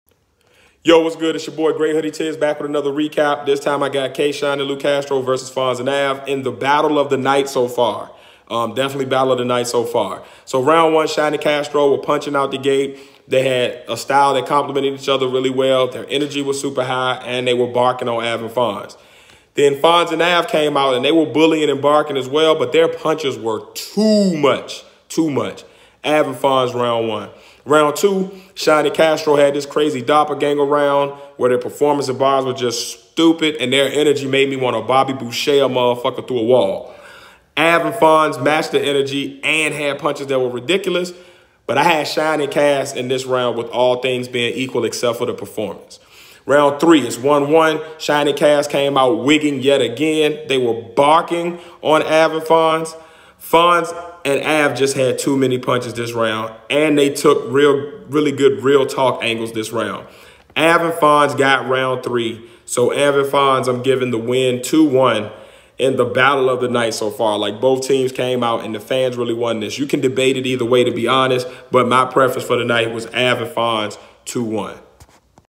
K Shine/Lu Castro vs Fonz/Ave 🔥🔥🔥🚨🚨🚨
Tiz recaps Shine and Castro vs Ave and Fonz right after the battle! This was a win for the fans all 3 rounds!!